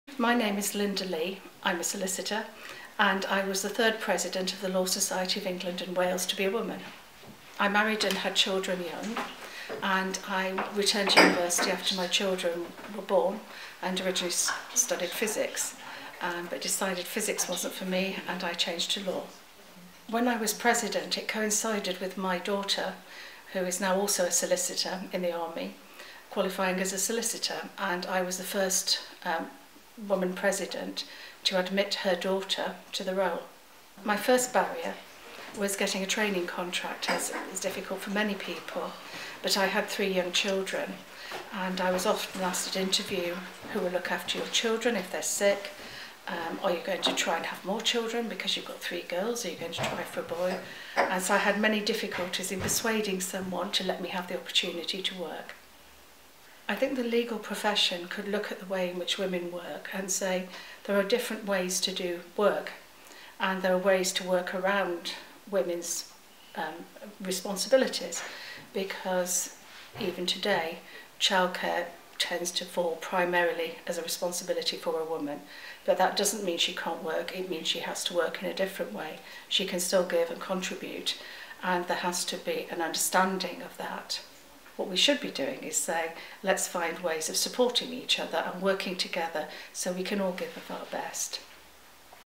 Linda Lee for First 100 Years
Linda Lee at the Association of Women Solicitors Summer Drinks 2017